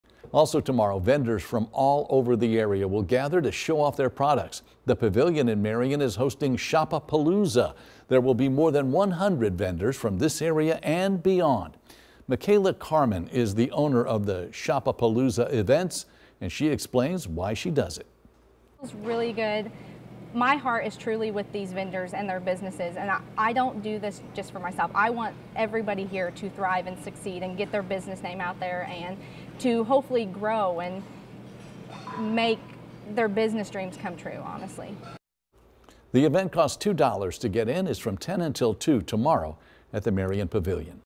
Local Shopapalooza underway in Marion
Vendors from around the area are gathering at the Pavilion in Marion.